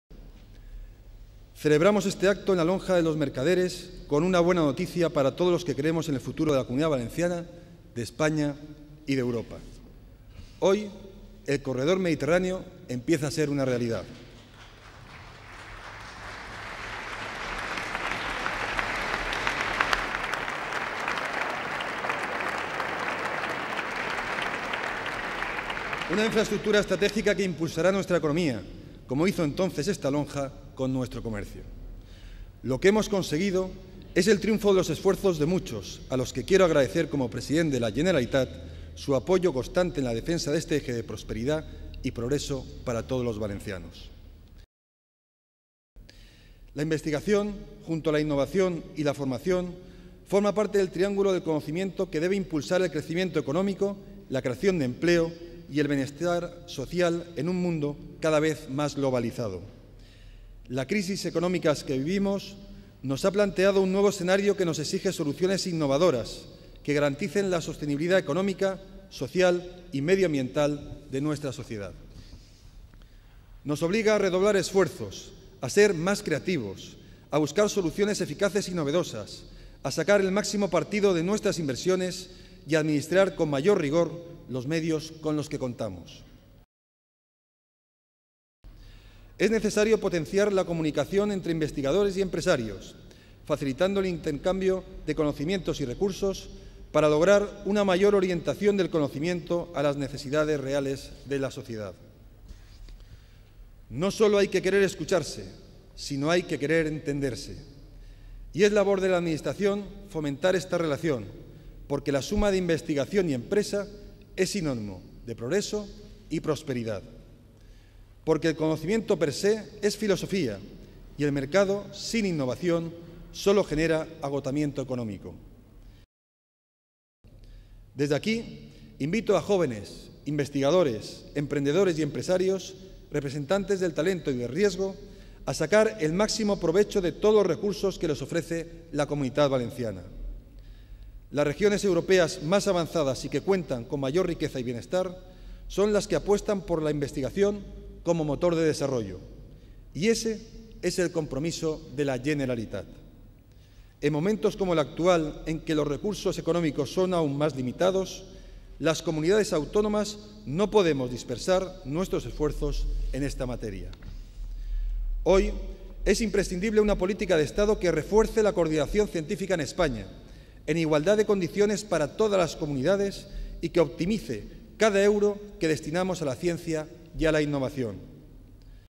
101911 Discurso del President de la Generalitat PREMIOS JAUME I.mp4
Discurso del President de la Generalitat, Alberto Fabra, con motivo de la entrega de los premios Rey Jaime I

El President de la Generalitat, Alberto Fabra, ha acompañado a S.M. la Reina Doña Sofía, en la visita que ha realizado al Hospital Universitario y Politécnico La Fe de Valencia.

La Reina Doña Sofía, además ha hecho entrega de los Premios Rey Jaime I en La Lonja y previamente visitó el Hospital Universitario y Politécnico La Fe de Valencia.